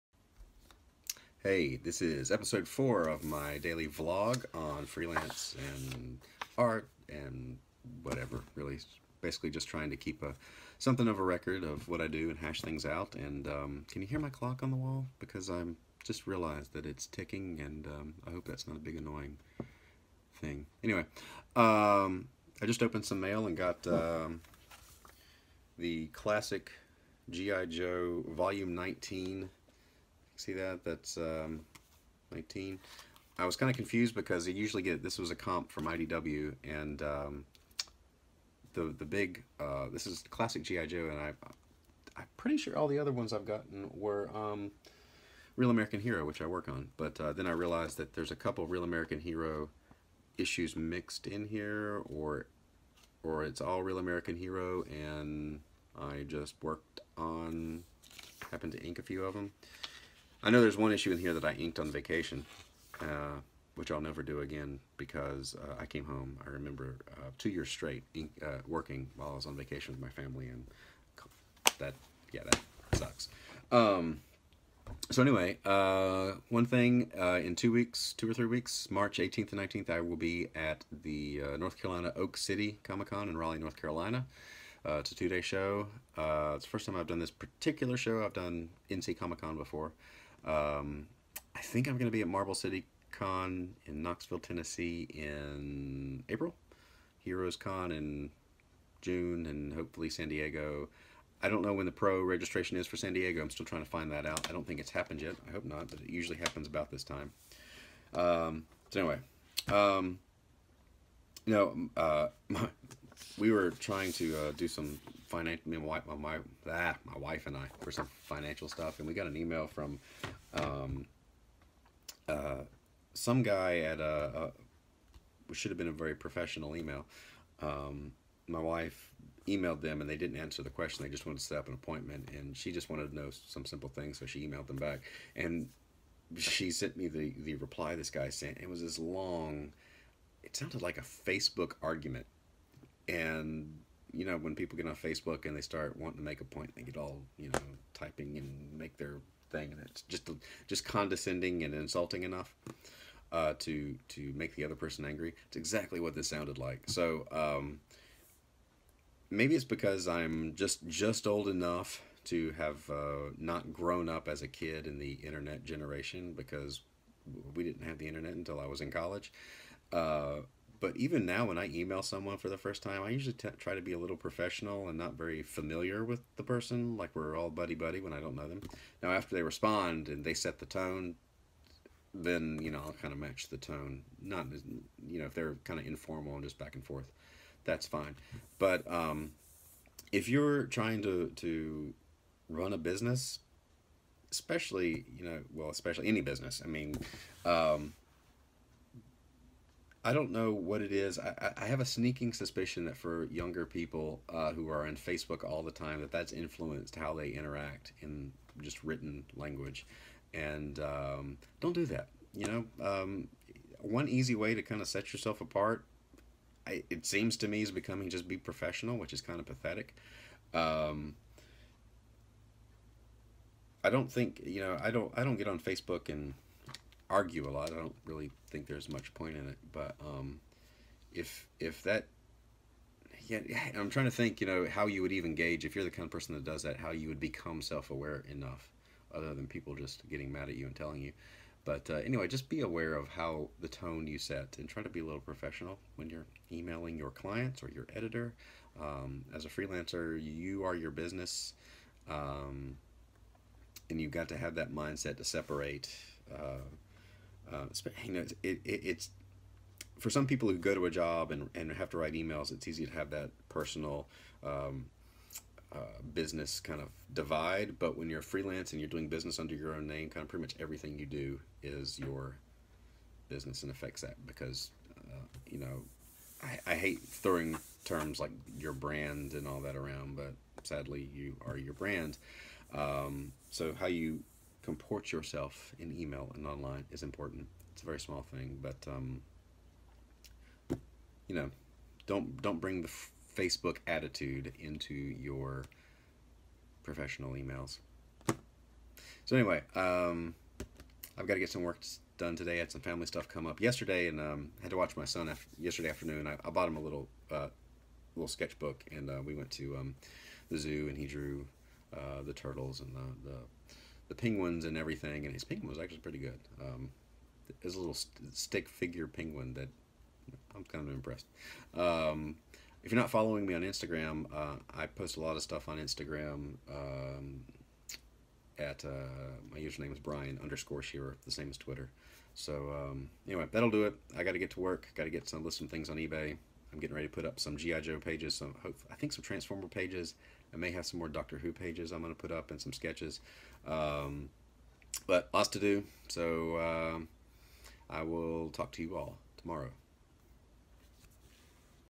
Keyboard Warriors vs. Professionalism | Brian's Daily Vlog - 004
Be careful not to get into social media debate mode when corresponding with clients/editors.